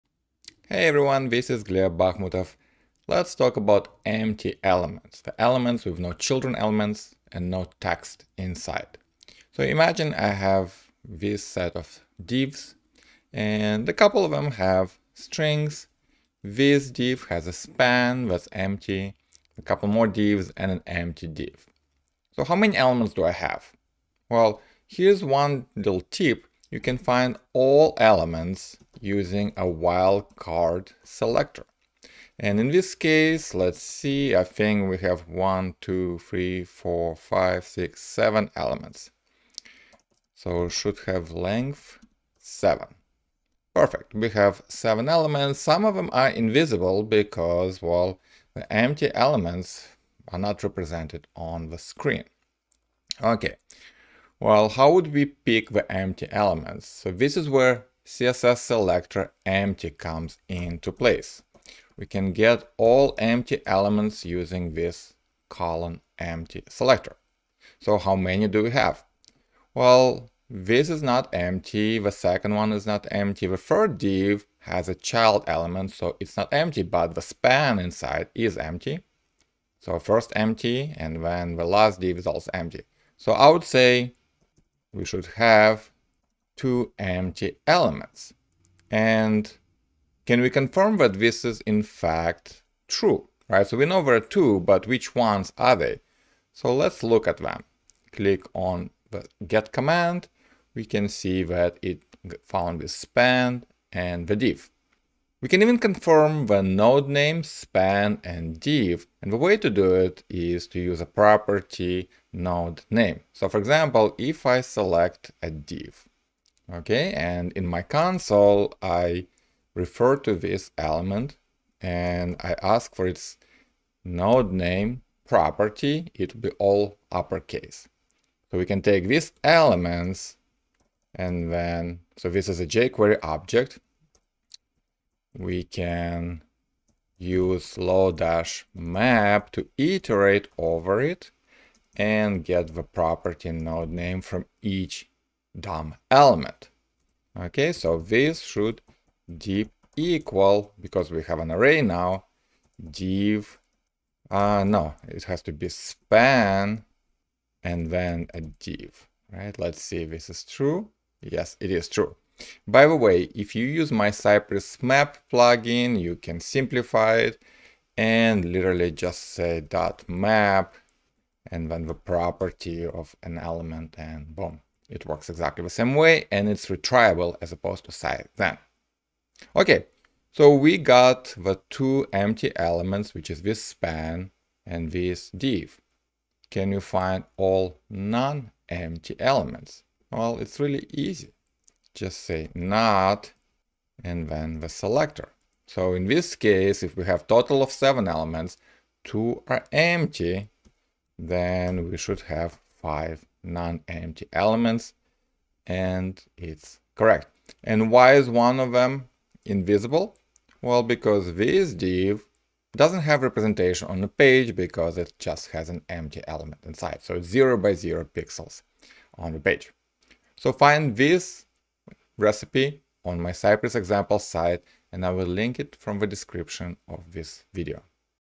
Empty And Non-empty Elements On The Page
We can find all empty elements on the page or its parts using ":empty" CSS selector. Empty elements have no children elements and no text inside. We can also find all non-empty elements by combining ":not(:empty)" selectors. recipe

// find all elements using wildcard selector
cy.get('*').should('have.length', 7)
// find all empty elements and confirm their node names
cy.get(':empty').should('have.length', 2)
  .map('nodeName')
 .should('deep.equal', [...])
// find all non-empty elements
cy.get(':not(:empty)').should('have.length', 5)